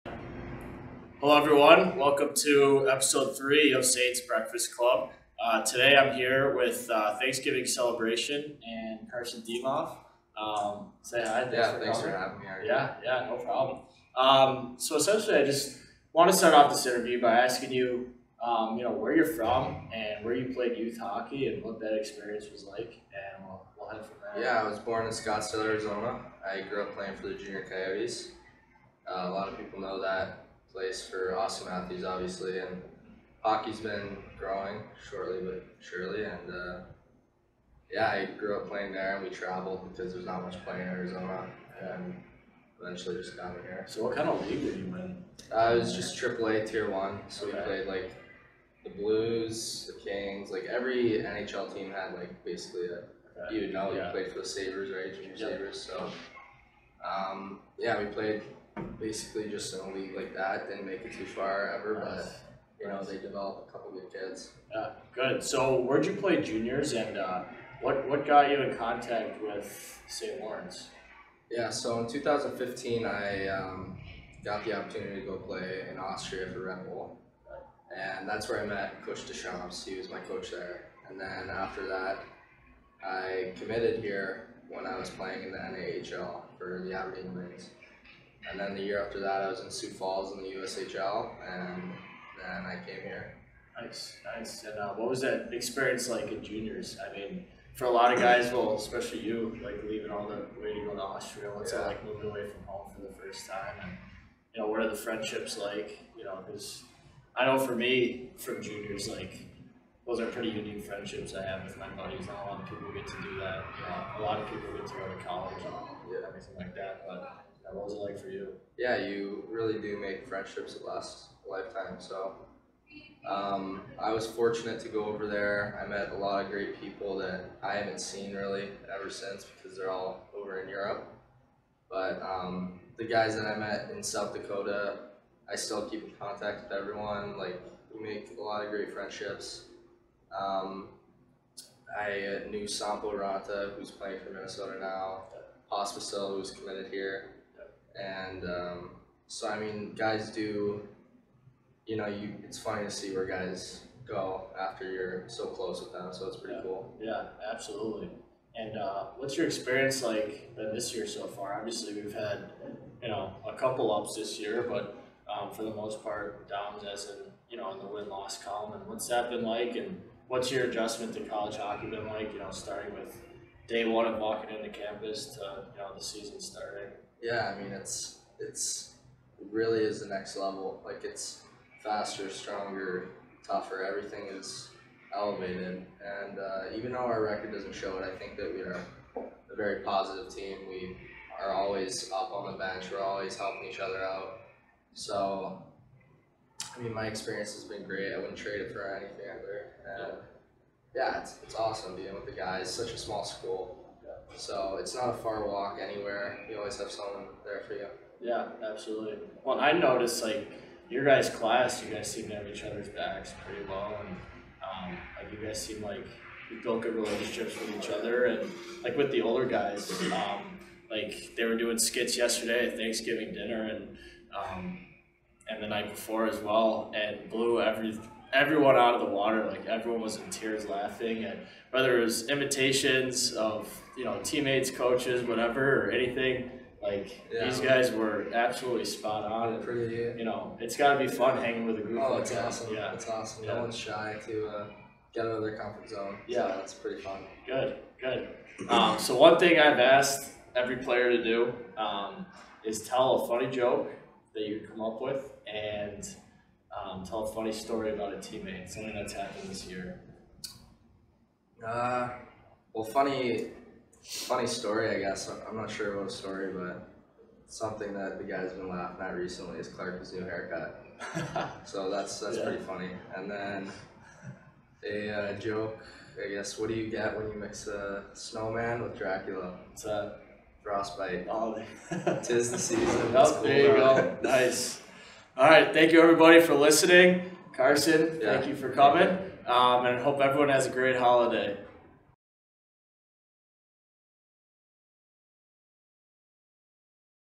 Saints Breakfast Club- EP3 Carson Dimoff
Episode 3 of the Saints Breakfast Club. Student Assistant Coach RJ Gicewicz checks in with first-year forward Carson Dimoff.